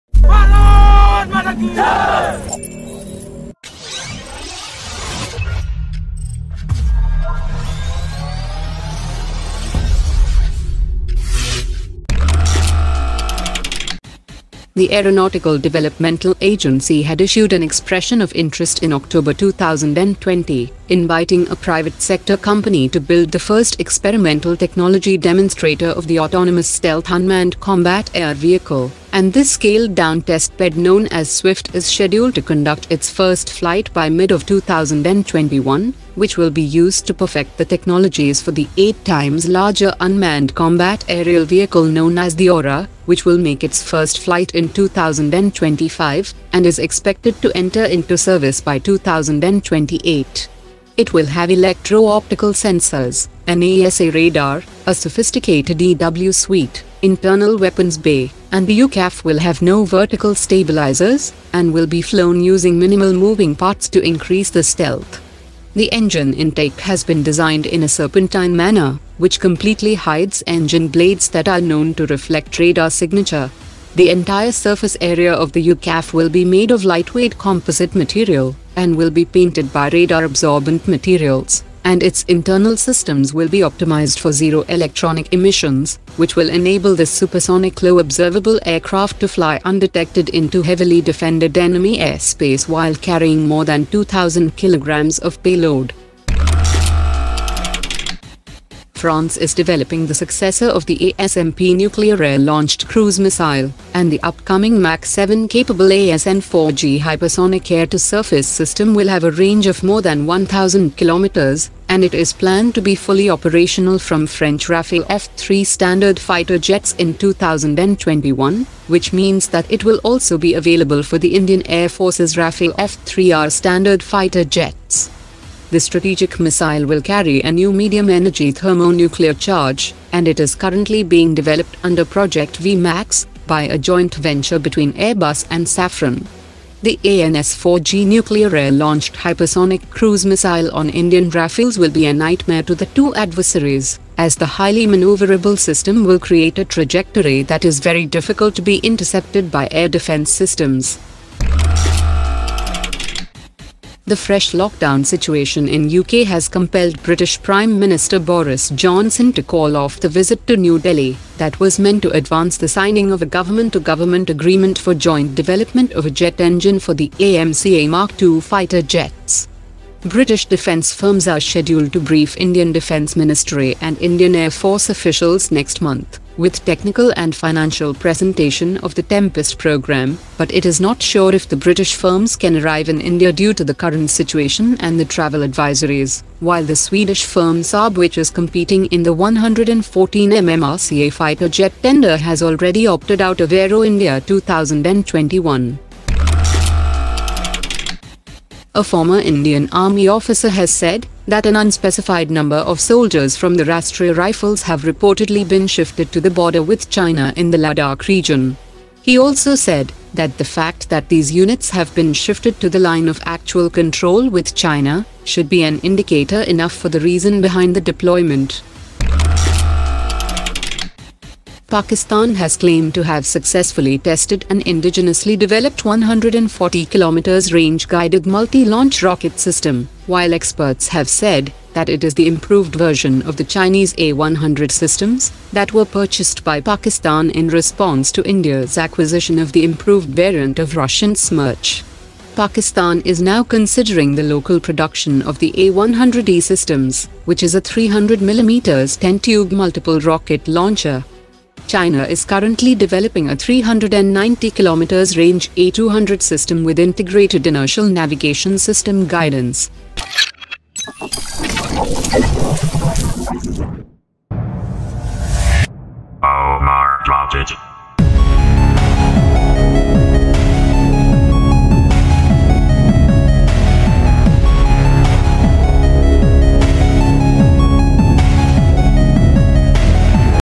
Indian Defence Updates : SWIFT UCAV 1st Flight,Mach 7 ASN4G For Rafale F3R,India-UK AMCA Engine Deal
1. SWIFTUCAV Testbed first flight scheduled In Mid-2021 : GHATAK aka AURA UCAV In 2024.
2. ASN4GRafale 1000 Km+ Mach 7 N.Capable Hypersonic ASN4G For Rafale F3 Ready In 2021.
3. UK PM Johnson delays India visit in setback for Indo-Pacific push : IndiaUKEngine Deal Tempest Briefing at AeroIndia2021 : SAAB out of AERO India 2021.
4. Sizable Indian soldiers from Rastriya Rifles move to China border : Analyst
5. Pakistan Claims to be test Fatah-1 system, with 140km range, Experts say improved Ch-inese A-100E.